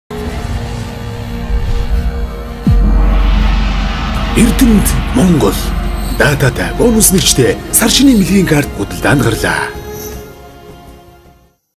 САР ШИНИЙН БАЯРААР 100% БОНУСТАЙ МОНГОЛ БЭЛГЭЭ СОНГООРОЙ
Юнител групп Монгол түмнийхээ хэрэгцээ, шаардлагад нийцүүлэн 1’000, 2’000, 3’000, 5’000 төгрөгийн тус бүр 100% бонус нэгж, дата эрхтэй ЭрдэмтМонгол САР ШИНИЙН БЭЛГИЙН КАРТ-уудыг худалдаанд гаргалаа. Урьдчилсан болон хосолсон, дараа төлбөрт үйлчилгээний бүх хэрэглэгч дансаа цэнэглэх боломжтой гэдгийг дуулгахад таатай байна.